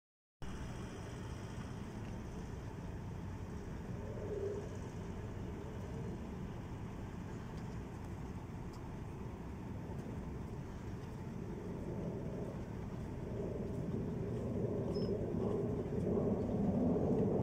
Большегруз застрял на подъёме у дома на Змеинке
Каждую зиму у жителей Змеинки во дворе застревают большегрузы, которые по навигатору едут в порт в районе бухты Анны. Фуры встают на крутом и скользком подъёме, а местные жители в это время не могут проехать к своему дому. Решить проблему мог бы дорожный знак, однако порт этого сделать не может и не хочет.